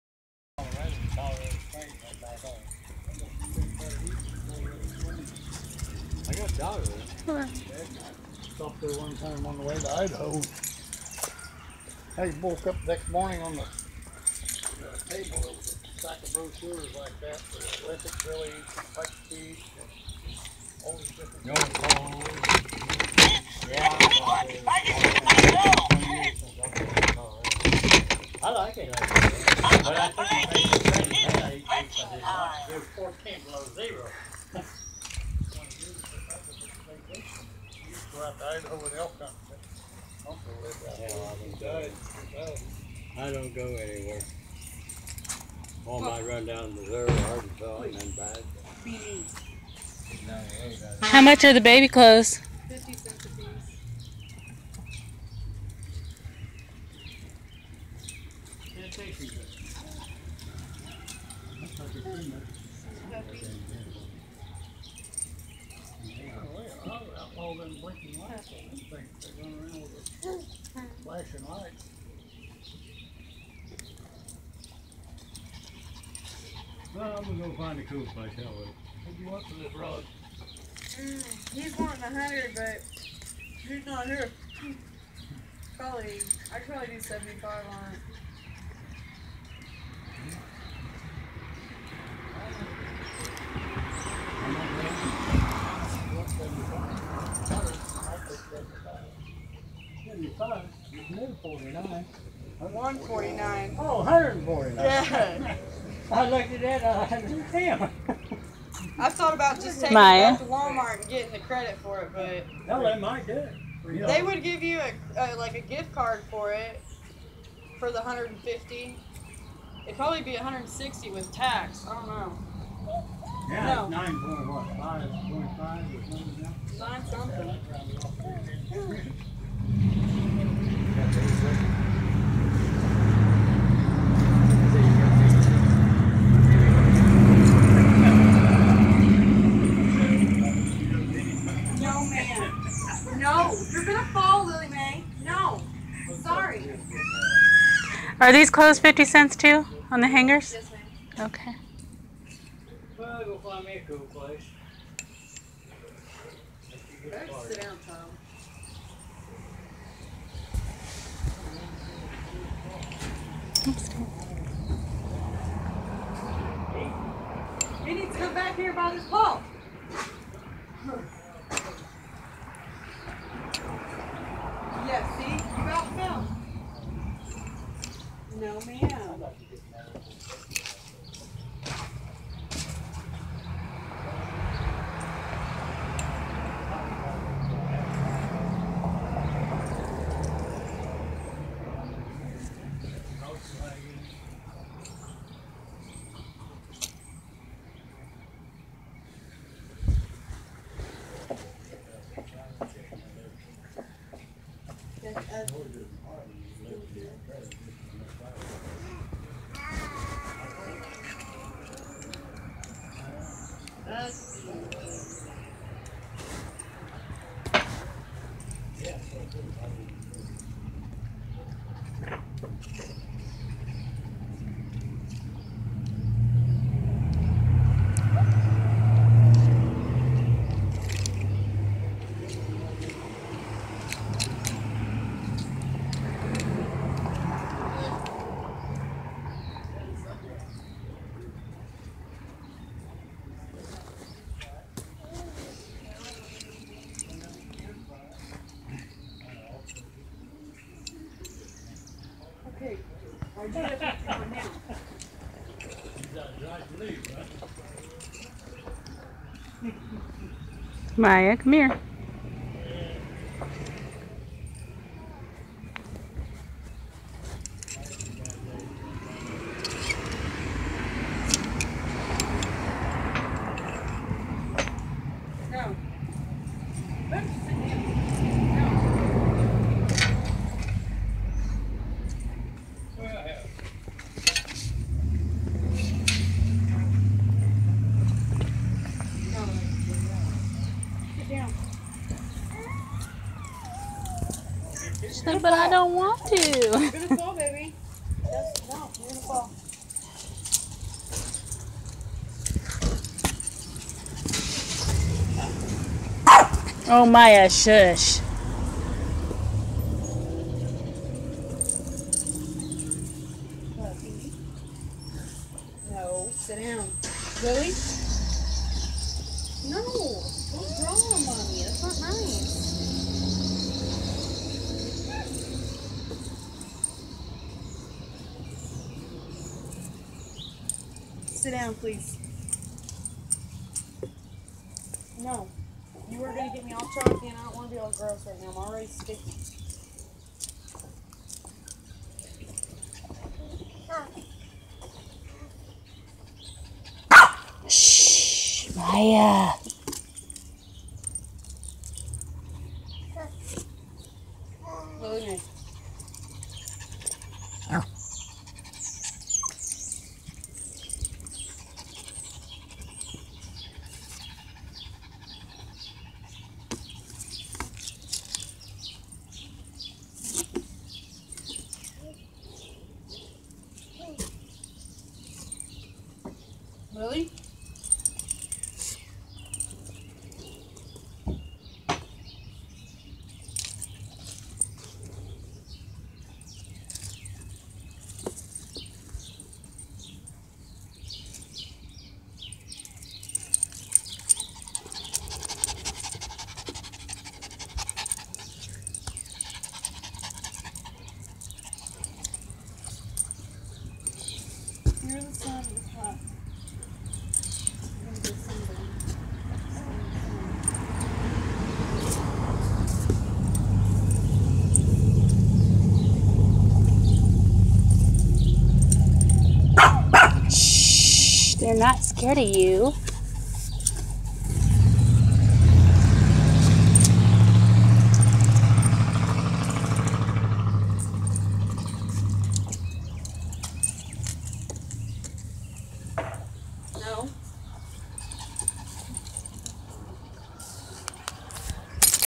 👶Yard sale shopping for the reborn babies!👶
Found a yard sale with a bunch of cute baby clothes!